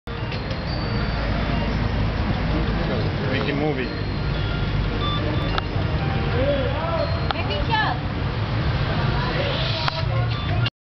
Anchalee Inn, patong - the bar with Meaow and an American Friend.AVI
A pick of the bar tendee named "Meaow" with an american friend.
At the bar of the Anchalee Inn, in Patong beach, Thailand, November 2010.